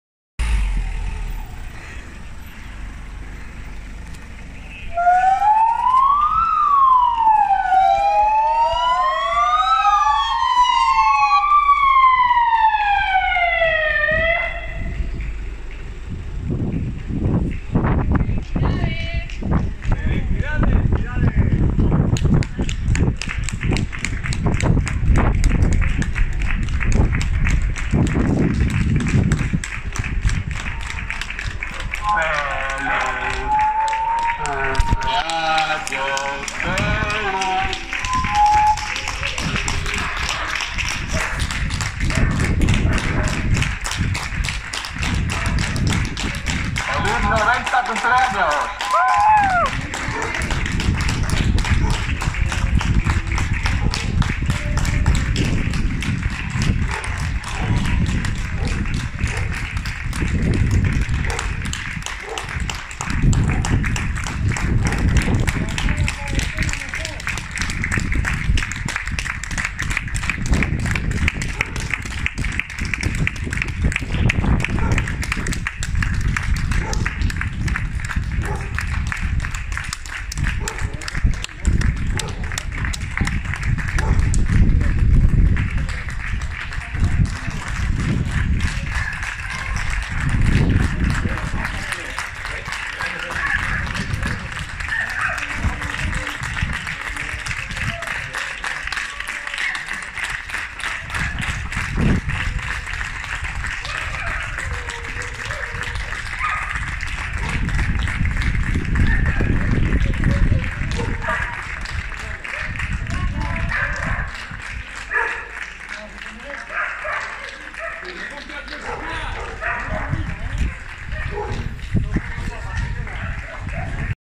Feliz cumpleaños a la abuela Josefina de parte de los PoliciaNacional de Santander
🥳Ayer nuestros compañeros fueron a felicitar a Josefina, una abuela que ayer cumplió 90 años y se encuentra sola con su cuidadora.
🎂Gran gesto por parte de este turno de PoliciaNacional en Santander que alegraron el día de su cumpleaños y le hicieron sentirse más acompañada